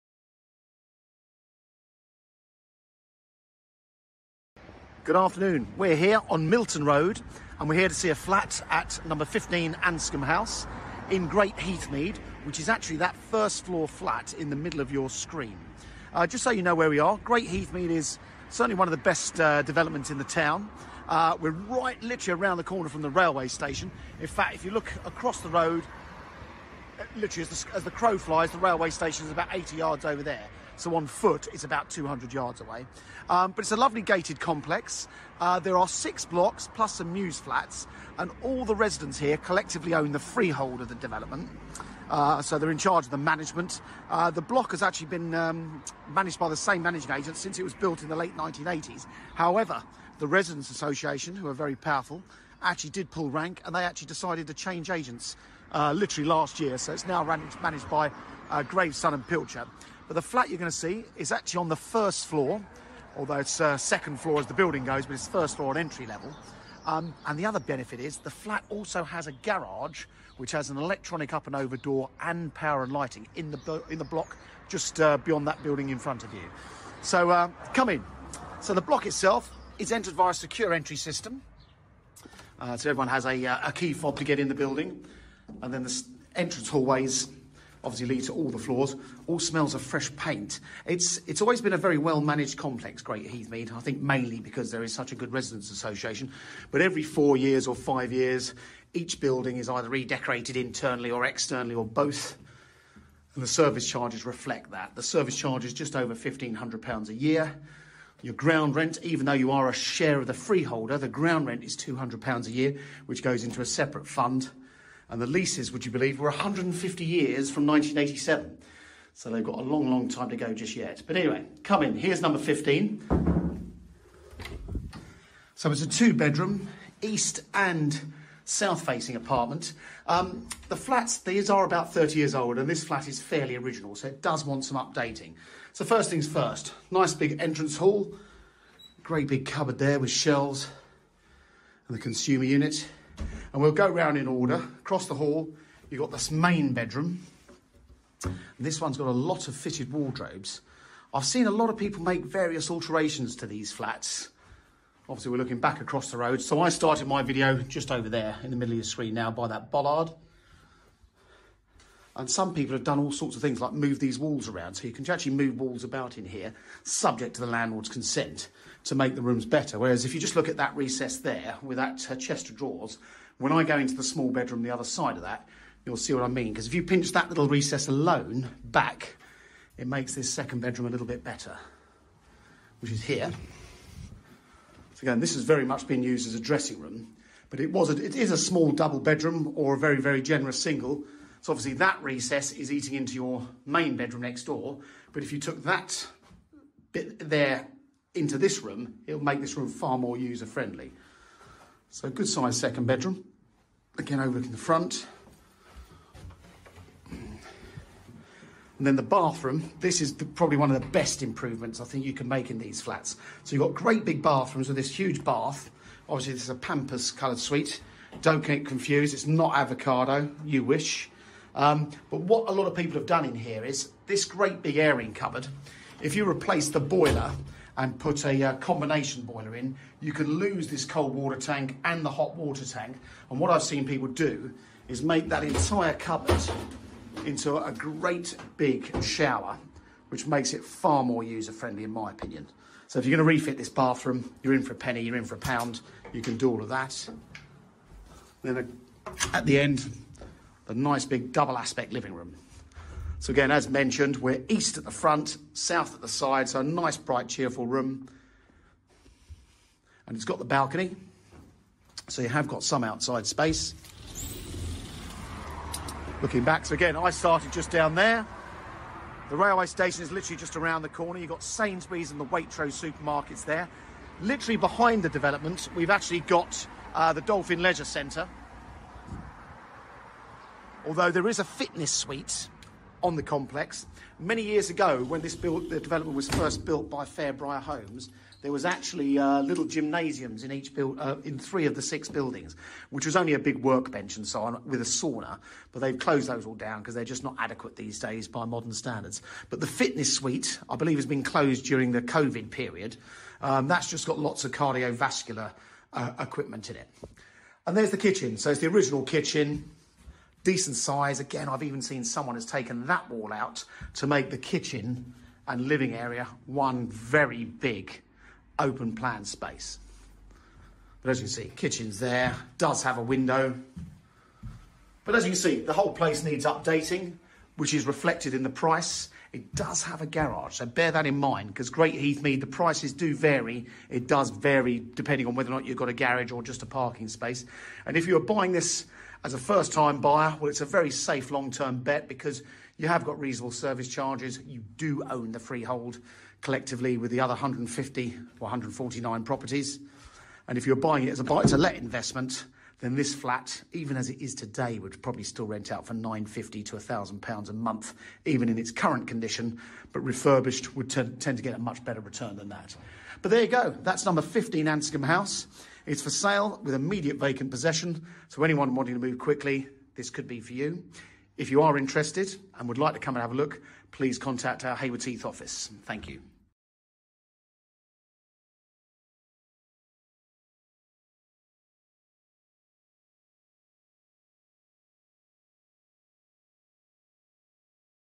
Anscome House
SALE AGREED
An online viewing of Anscome House, on the market at £260,000.